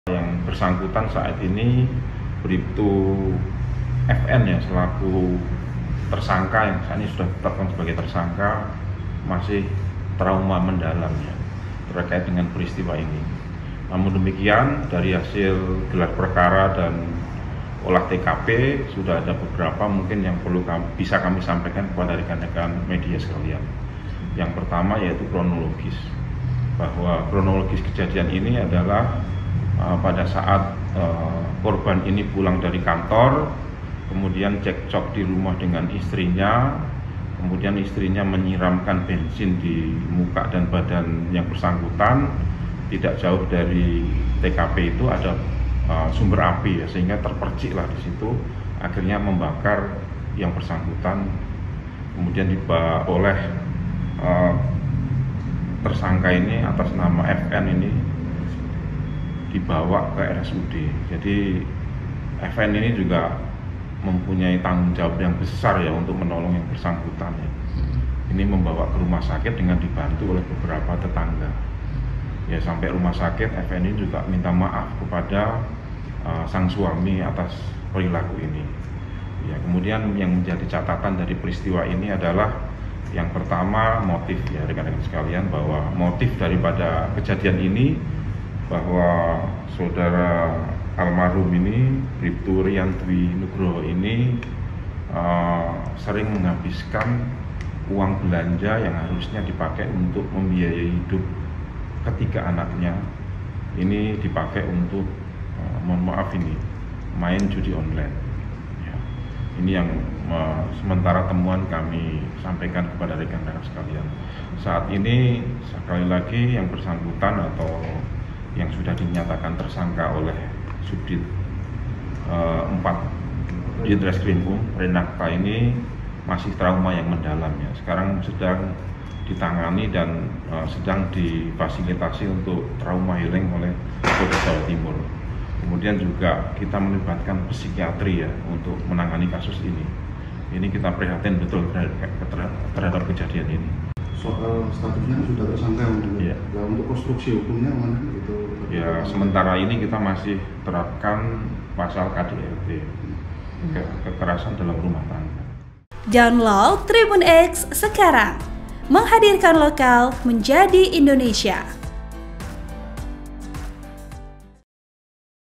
POLISI BAKAR SUAMI Minta Maaf, Mengaku Kesal Uang Habis Untuk Judi Online!
TRIBUN-MEDAN.COM - Kabid Humas Polda Jatim, Kombes Dirmanto menyebut Briptu FN yang sudah ditetapkan tersangka juga membawa ke rumah sakit dan sempat meminta maaf kepada suaminya.

Hal ini disampaikan Dirmanto saat ditemui di Surabaya pada Minggu (9/6/2024).

Darmanto mengatakan Briptu FN membawa suaminya usai dibakar ke rumah sakit dengan bantuan para tetangga.

"Sampai rumah sakit, FN juga minta maaf kepada sang suami atas perilaku ini," ujar Dirmanto.

Diketahui, tersangka nekat membakar suaminya lantaran kesal Briptu RDW kerap menghabiskan uang untuk judi online.

Editor Video : M Andimaz Kahfi